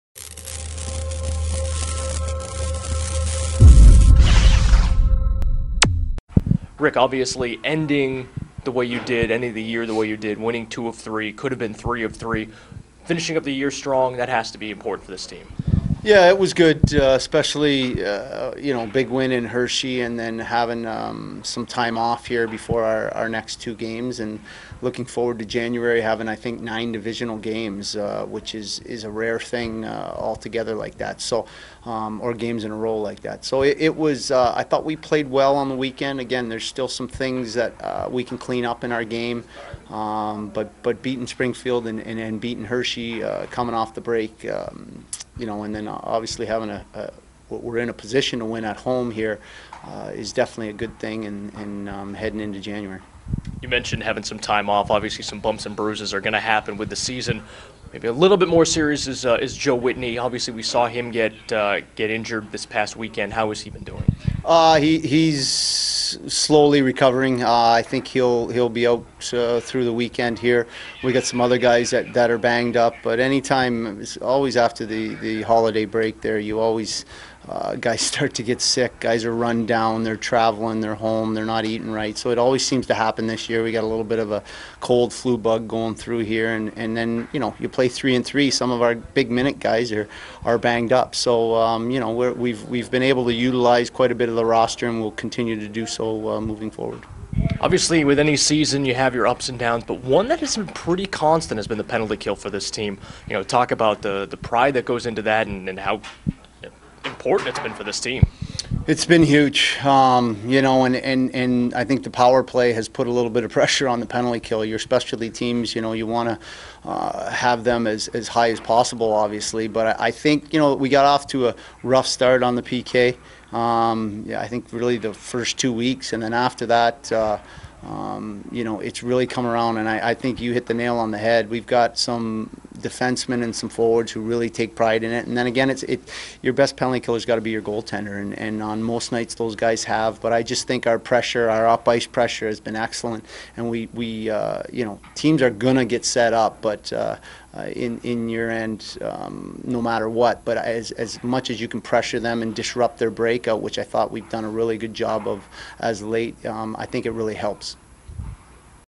1.2.2012 Rick Kowalsky Interview
Head Coach Rick Kowalsky discusses the team's most recent weekend, injuries and the penalty kill unit.